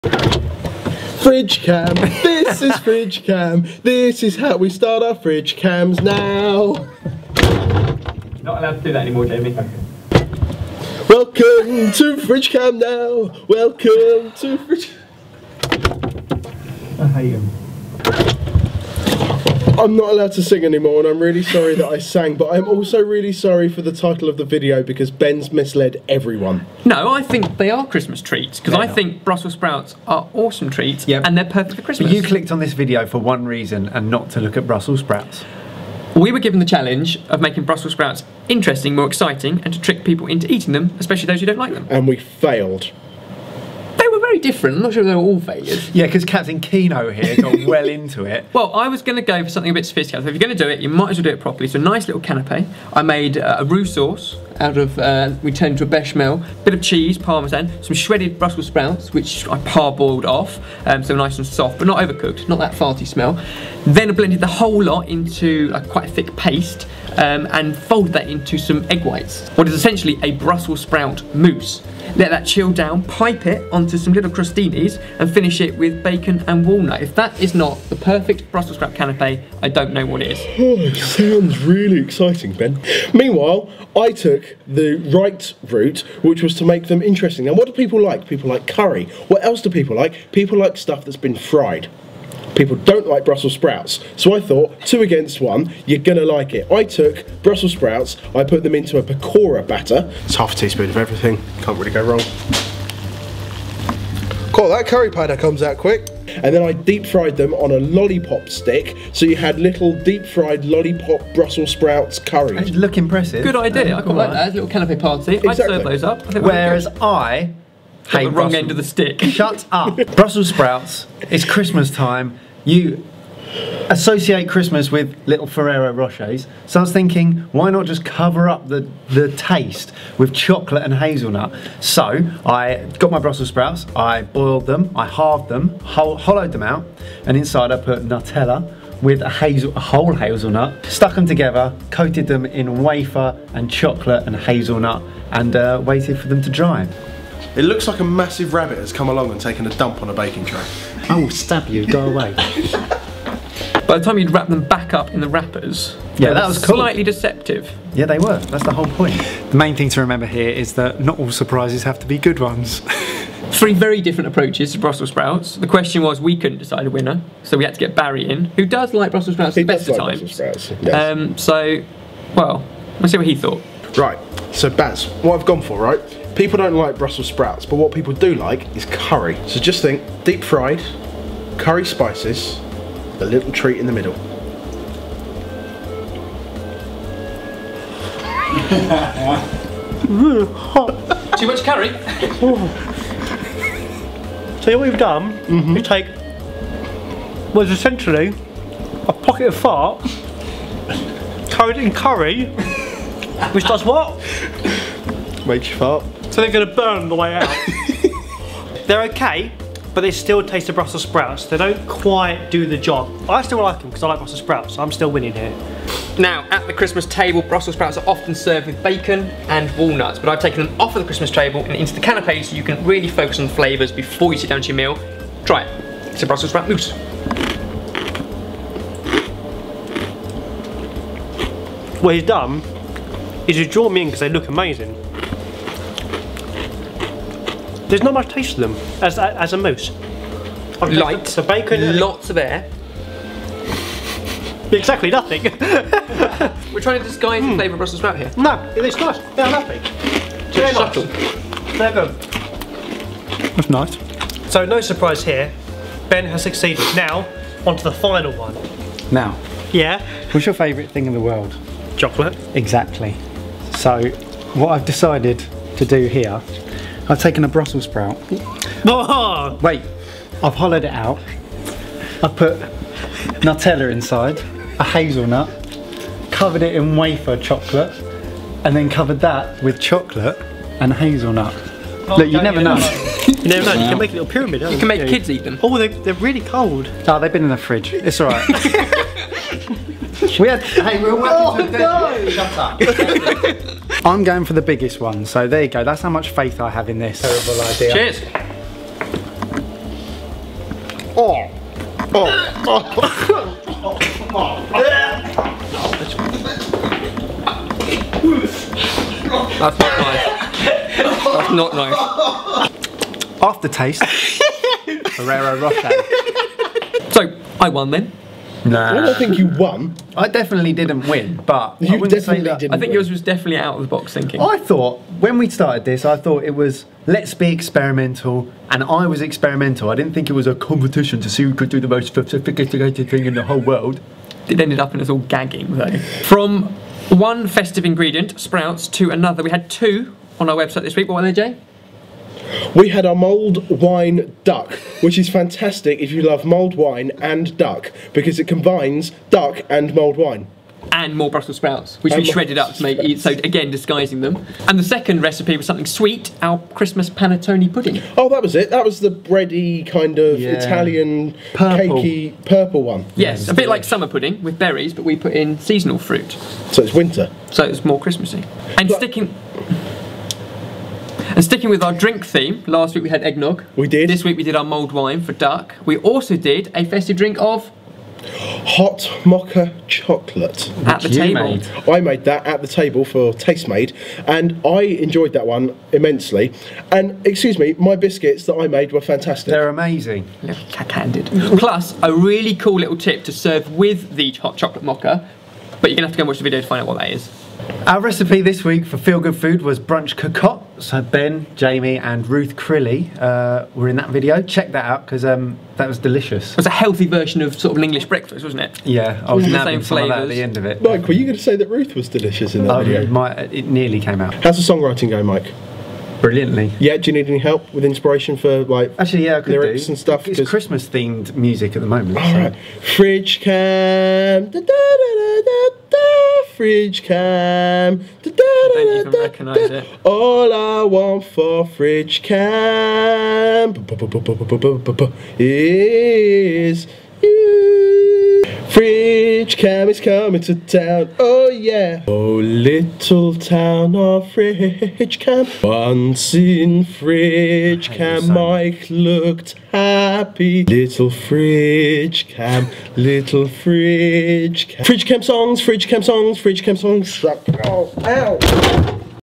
Surprise Xmas Treats | Sorted Food
Christmas carol-okie from Jamie and 3 ways with Brussel Sprouts! Ben might have taken the challenge of cooking something interesting with sprouts a little too seriously... but Jamie takes the right approach. Not sure what the hell Mike was thinking...

Don't forget to check out the two new YouTube series episodes we've made: